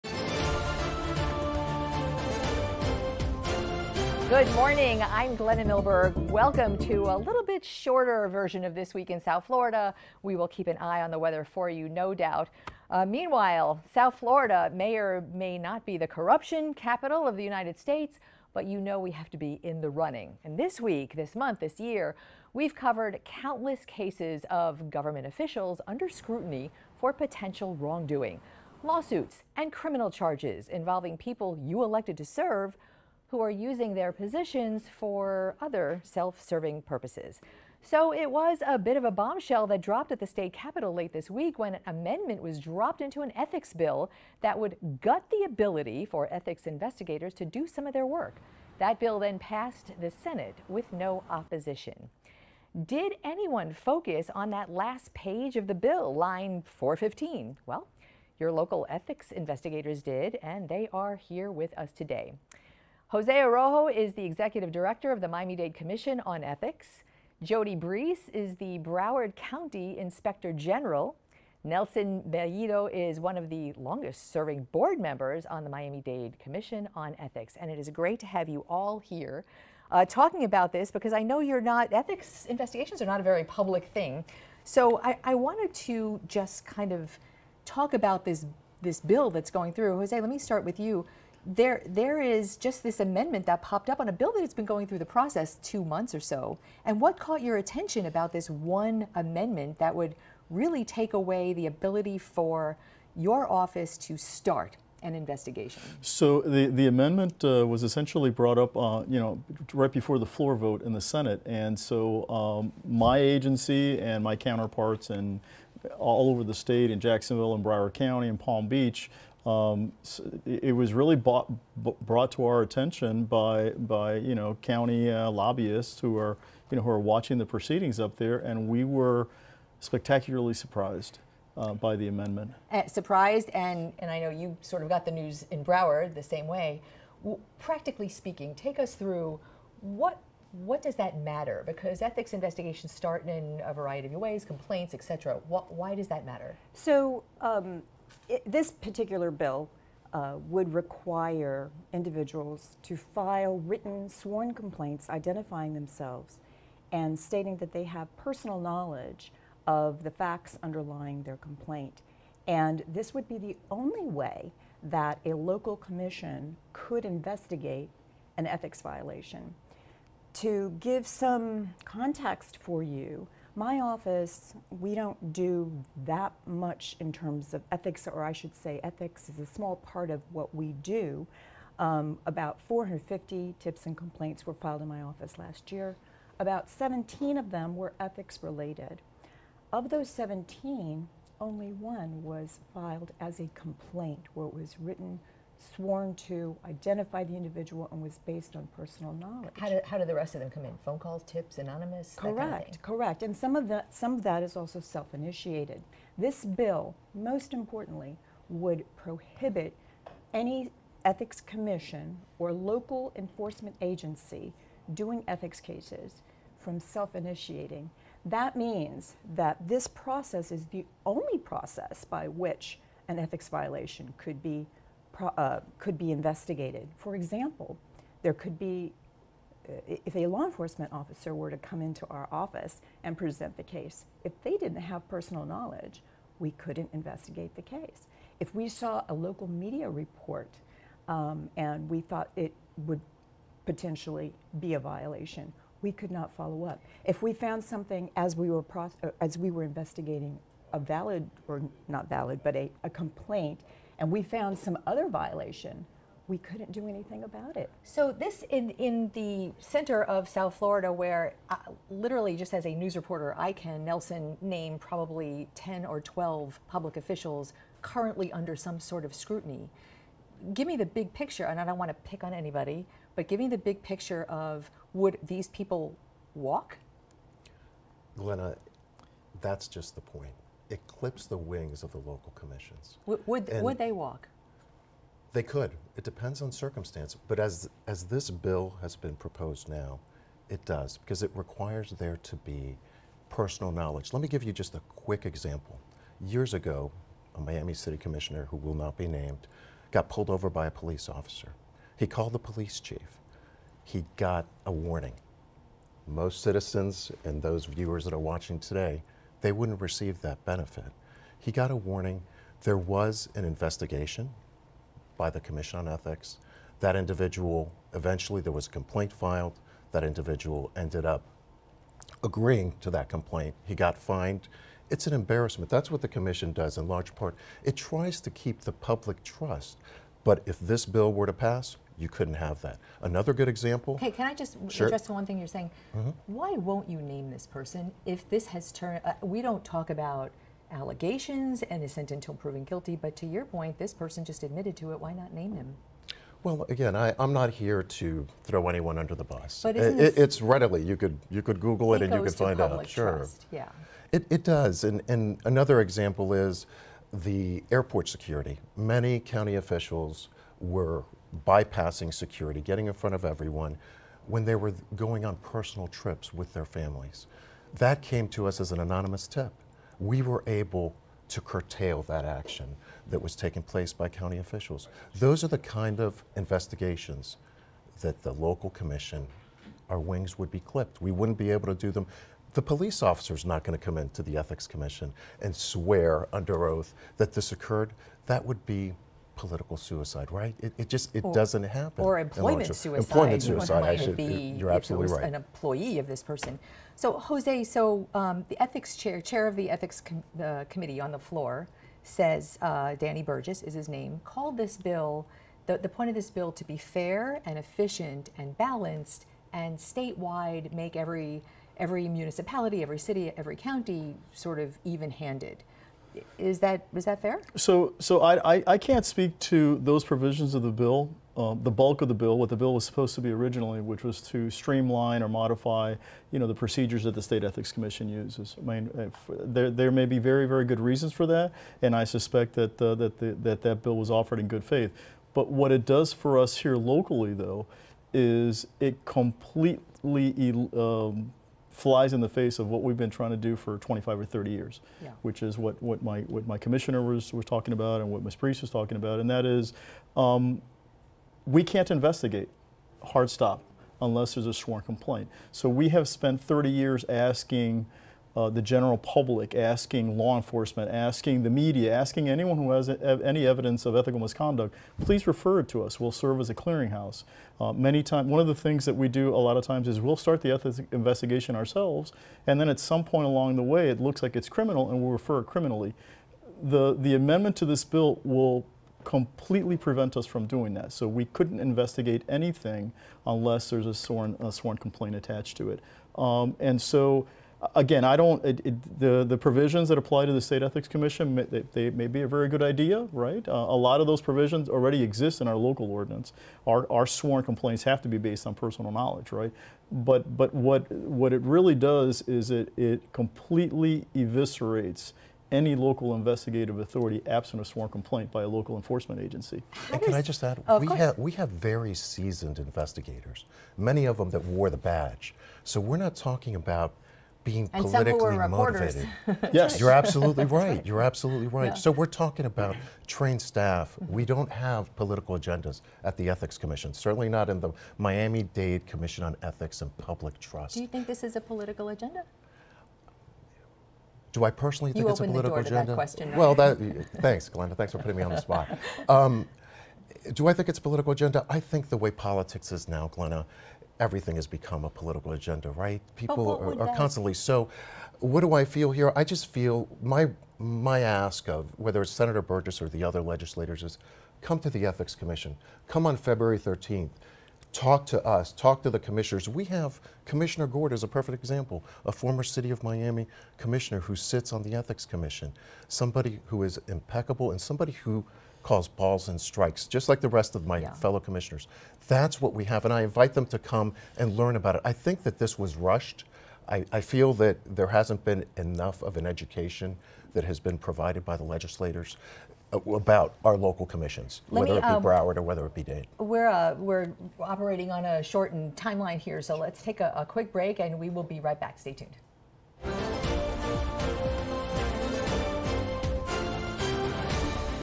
Amendment to ethics bill weakens public corruption investigations
Miami-Dade Ethics Commission Executive Director Jose Arrojo, Broward County Inspector General Carol Breece, and Attorney Nelson Bellido, an Ethics Commission Board member.